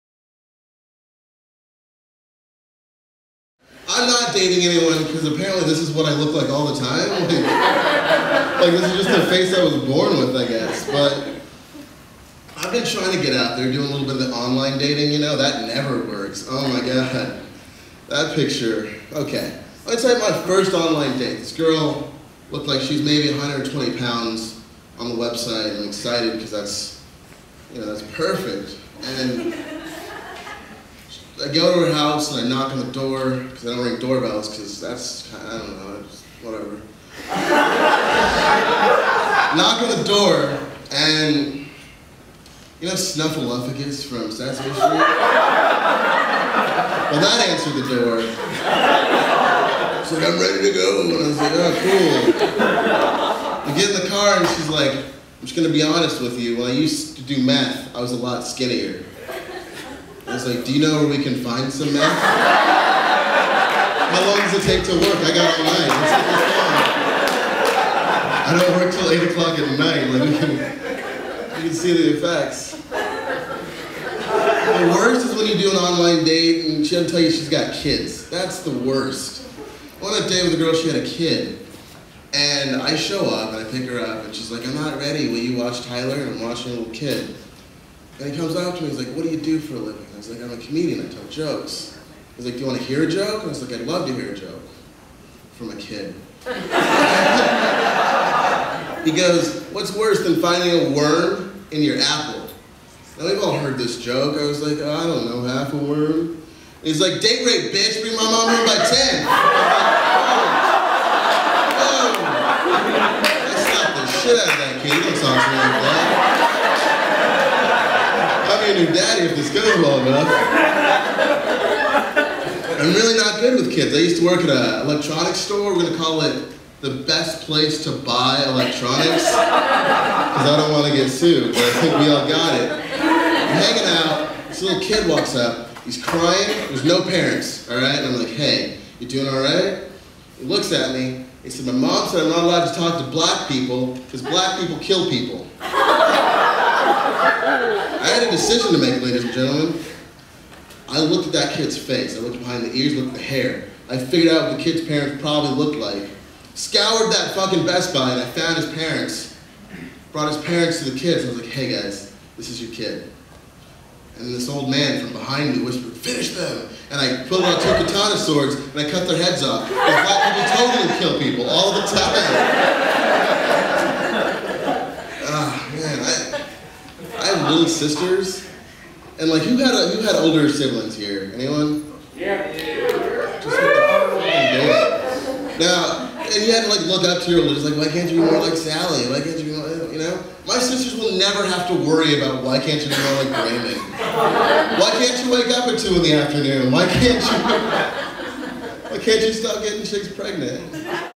Dating and Children
A video from the aborted DVD show, pretty funny clip about dating and kids. Enjoy!